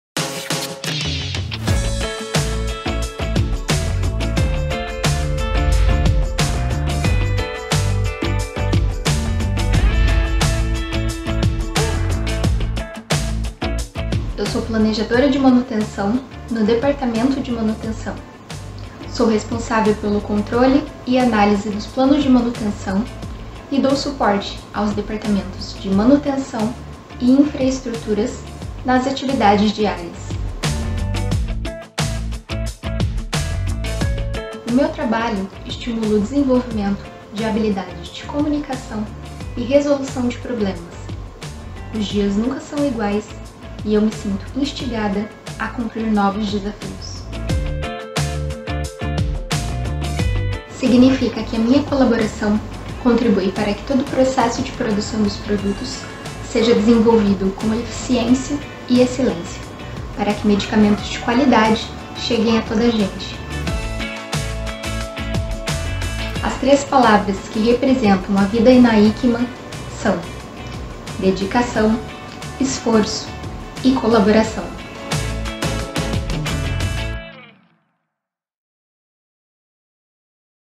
​OwnYour Career | Janaina Cristina Ferreira, Maintenance Planner, Engineering. Portugal
Meet Janaina, Maintenance Planner, Engineering at Hikma Portugal. She loves tackling new challenges every day and using her communications and problem solving skills on the job.